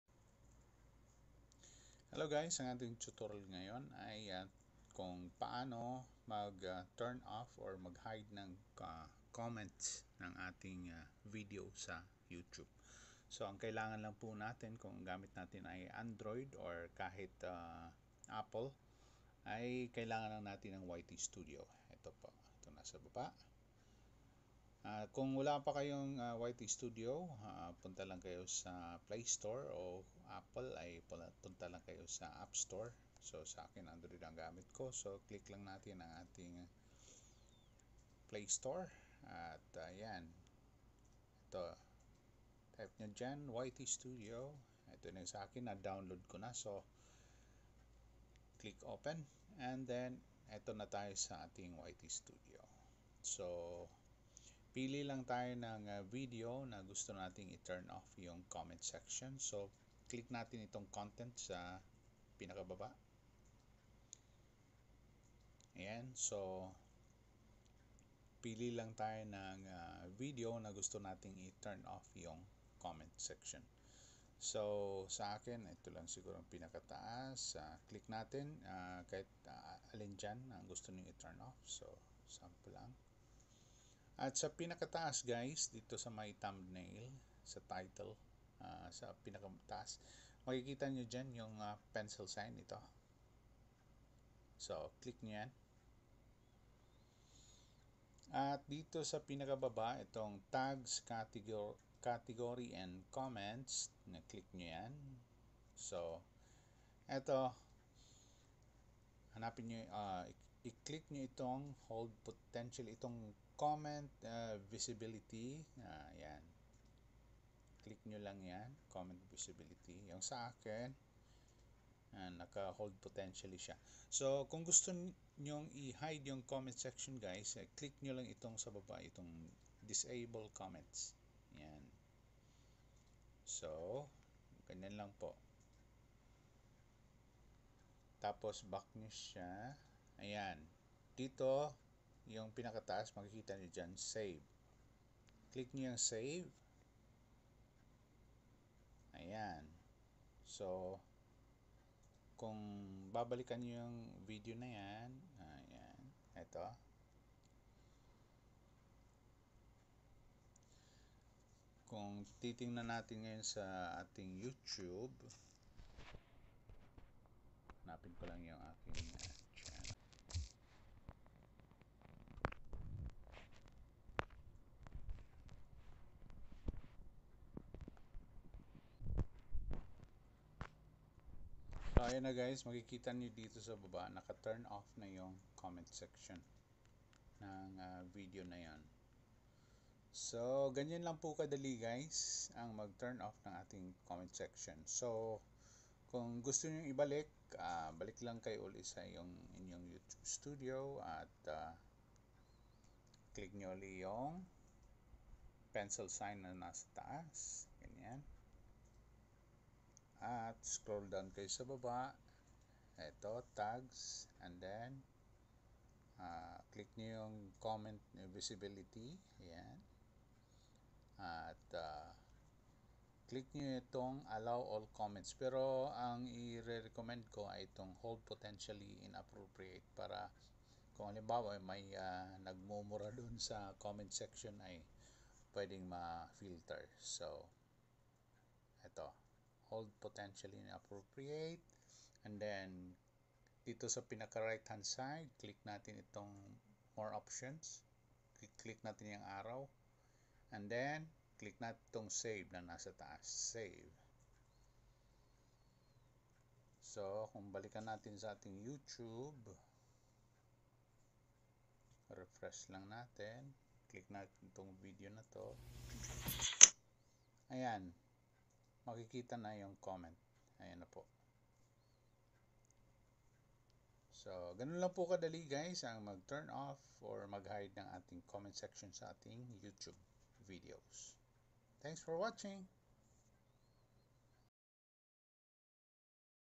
How to hide/unhide comment section on YouTube | android
Paano mag hide or unhide ng comment section sa youtube